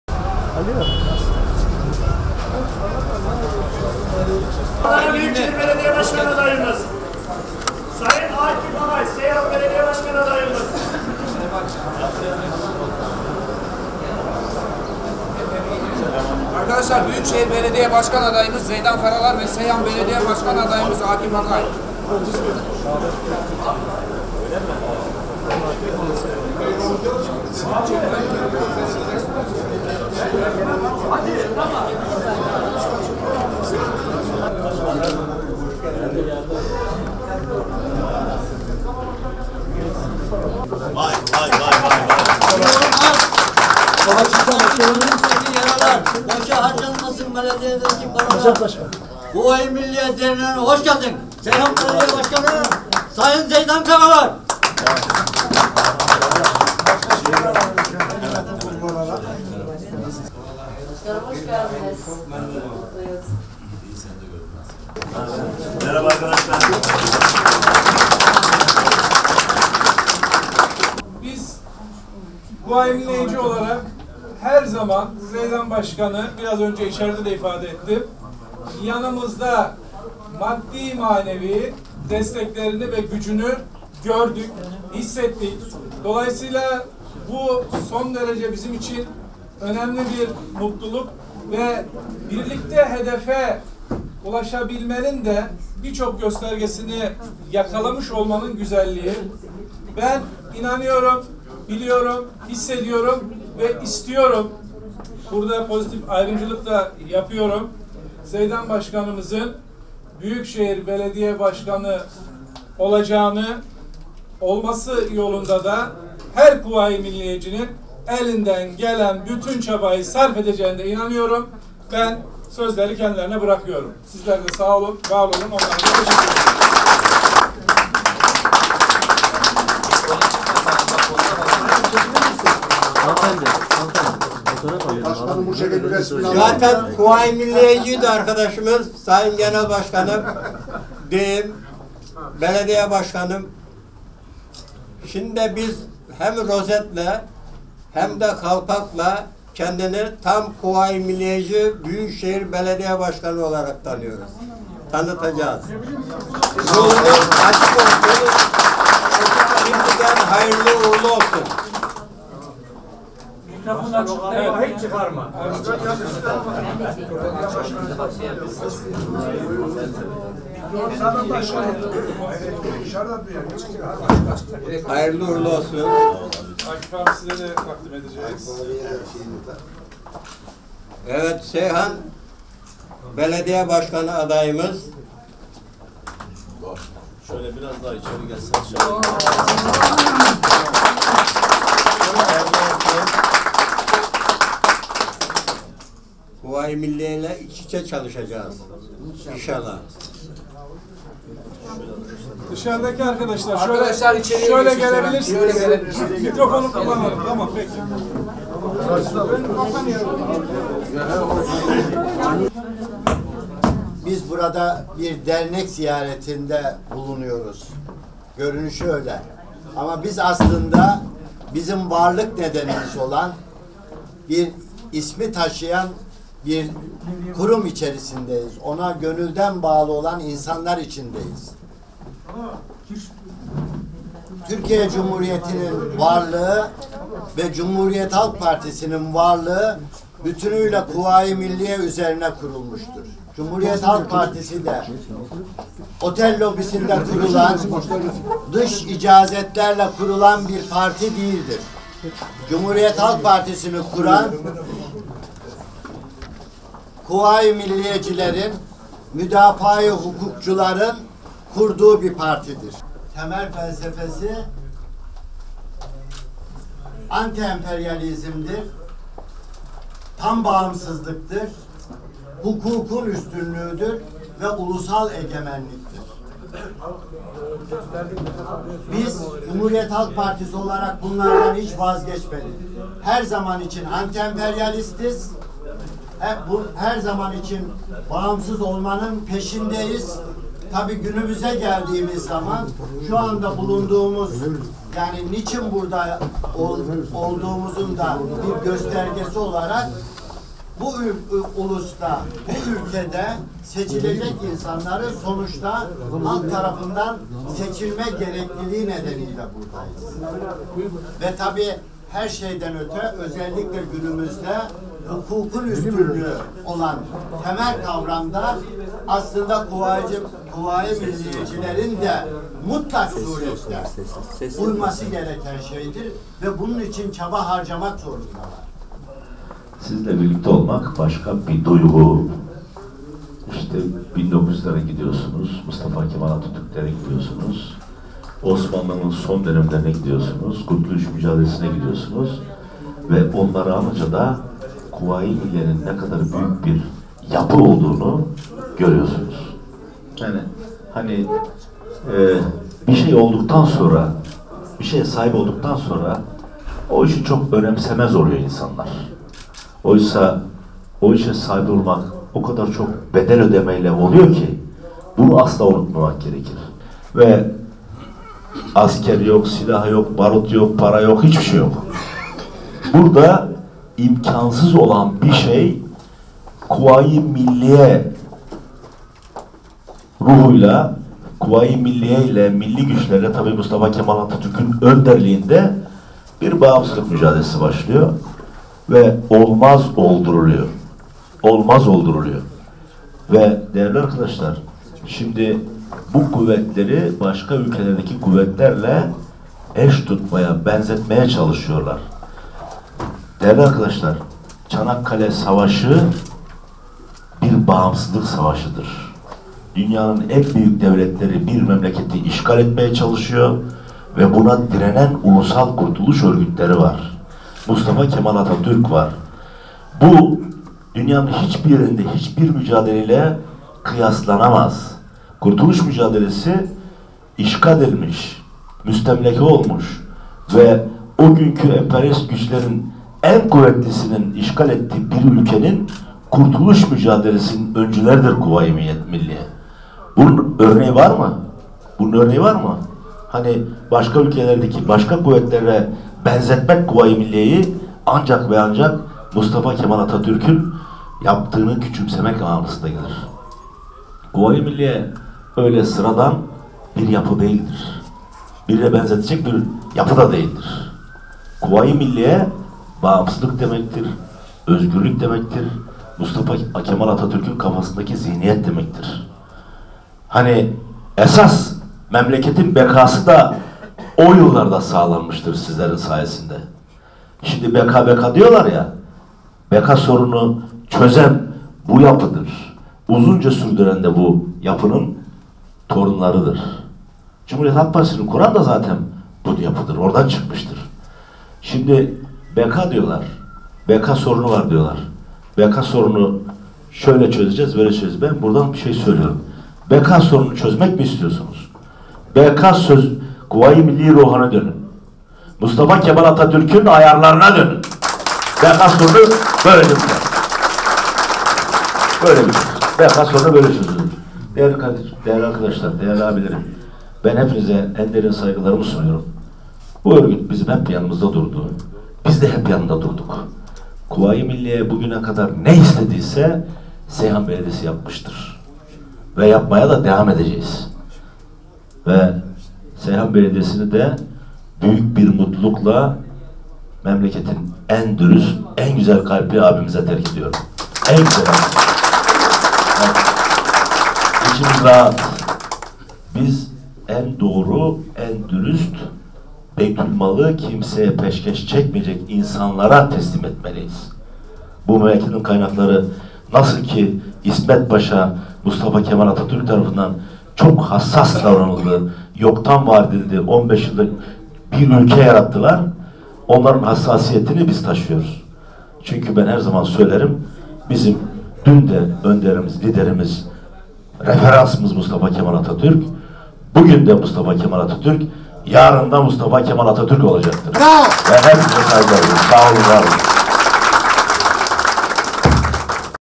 Karalar: Beka Sorunu Diyenler Atatürk Ayarlarına Dönsün
Seyhan Belediye Başkanı ve Cumhuriyet Halk Partisi Adana Büyükşehir Belediye Başkan Adayı Zeydan Karalar ile CHP Seyhan Belediye Başkan Adayı Akif Kemal Akay Kuvayi Milliye Mücahitler Derneğini ziyaret etti.